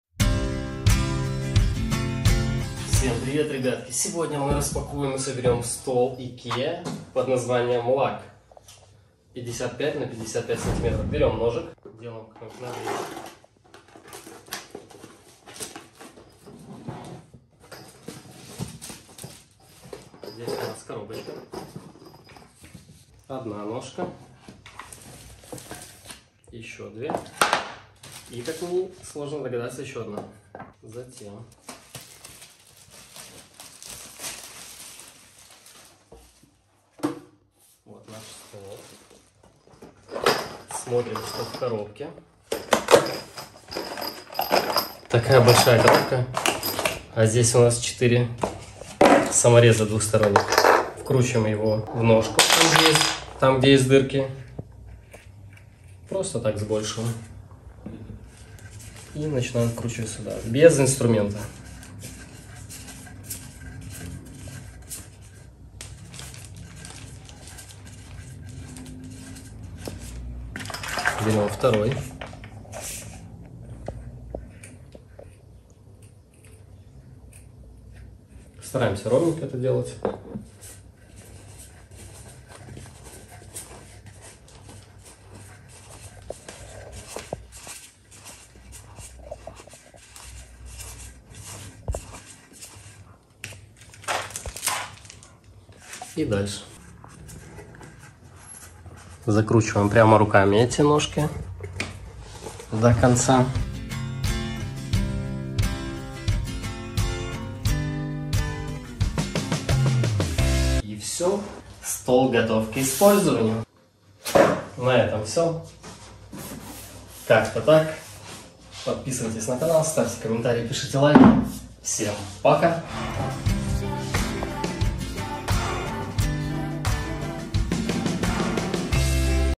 СТОЛ IKEA LACK 55х55 см Как собрать Сборка мебели ИКЕА 304.499.08
Конструкция стола с сотовидным бумажным наполнителем обеспечивает его легкость и устойчивость одновременно. Его легко привезти из магазина, установить там, где вам удобно, и перемещать при необходимости.